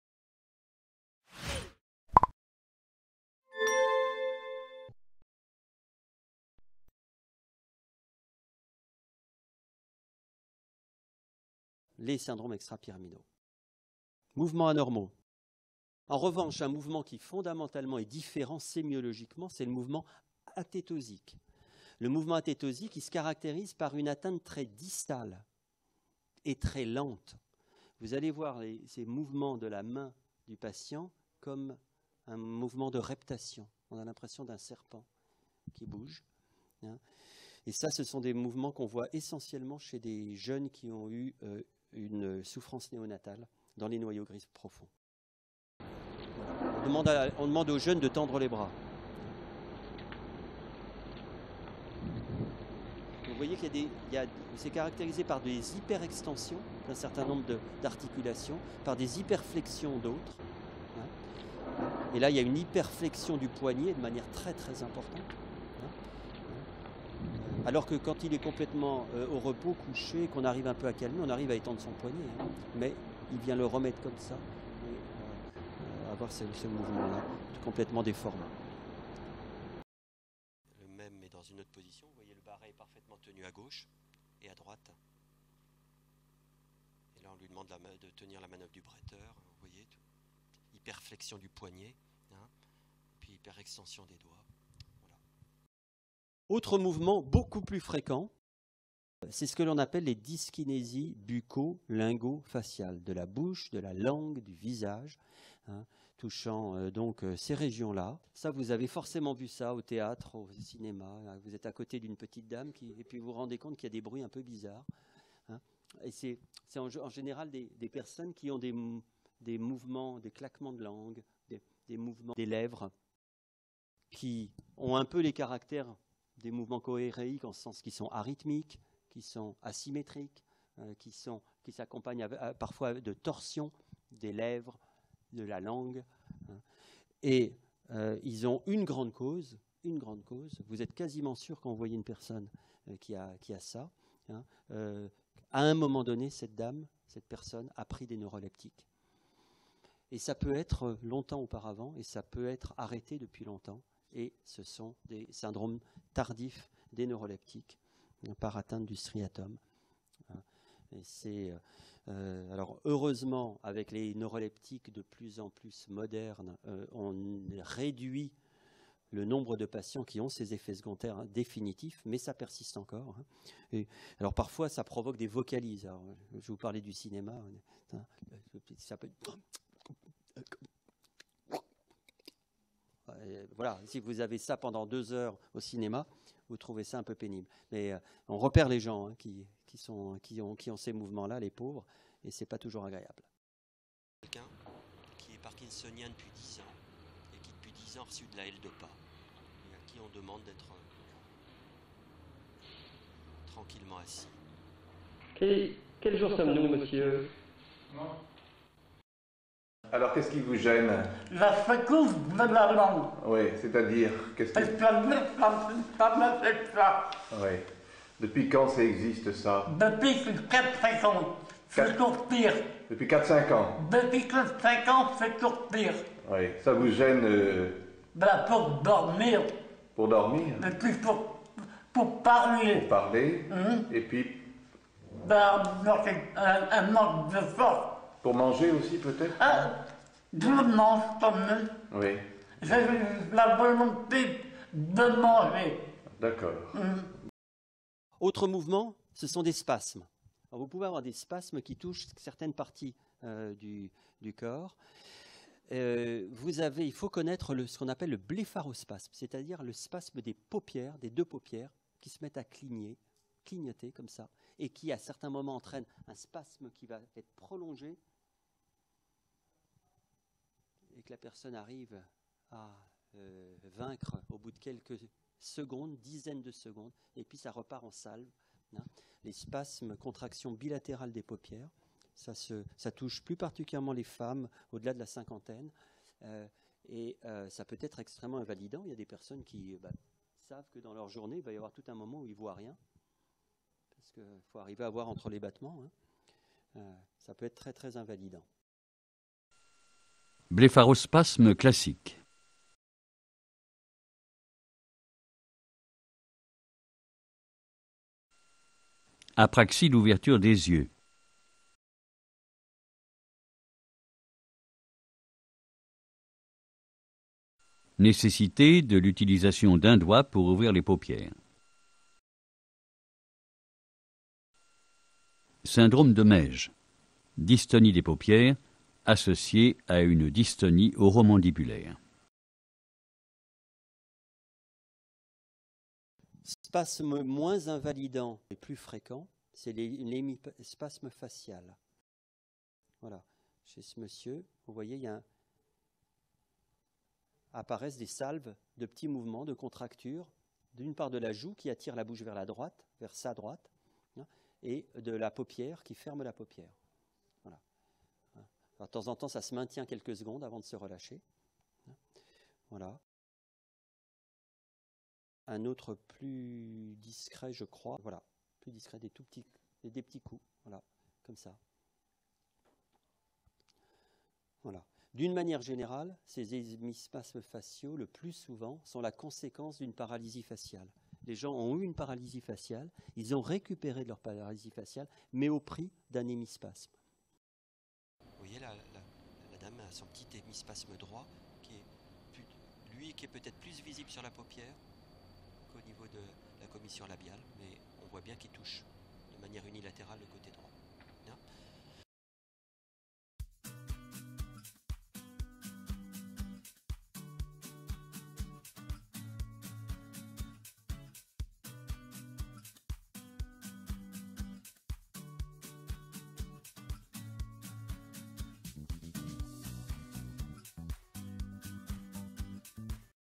Syndromes Extra-Pyramidaux - Mouvements Anormaux (2ème Partie)
On appelle mouvements involontaires ou mouvements anormaux involontaires une activité motrice indépendante de la volonté, survenant à l'état de veille.
Pour la plupart d'entre eux, mais pas pour tous, ils résultent d'un dysfonctionnement ou d'une lésion des noyaux gris centraux.
Il en existe deux grandes variétés : certains sont rythmés et réguliers (tremblements), d'autres sont non rythmés.

PLAN :
00:00 Channel Intro
00:11 Mouvement Athétosique
01:36 Dyskinésies Bucco-lingo-faciales
04:07 Blépharospasme
05:35 Hémispasme facial